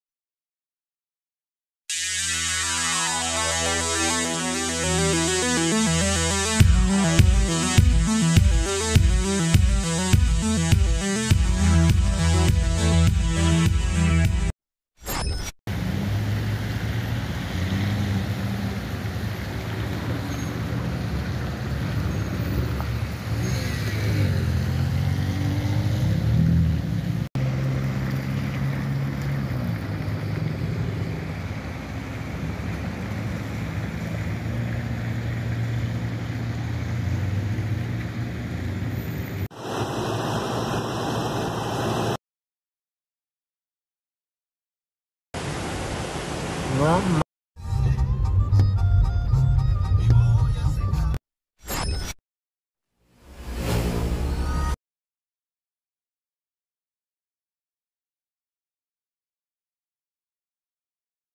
lluvia matutina colapsa a la ciudad de Guadalajara
Los canales pluviales de el municipio de Tlajomulco totalmente colapsados por los altos niveles de agua, 50 casas afectadas por inundaciones y al menos 15 autos varados el saldo de la lluvia de este viernes en al área metropolitana de Guadalajara